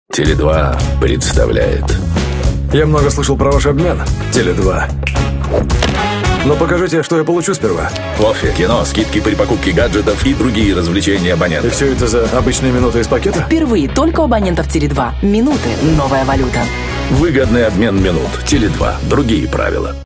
ТЕЛЕ2: Минуты- новая валюта. Выгодный обмен минут (2023)
Реклама услуг и тарифов оператора сотовой связи ТЕЛЕ2.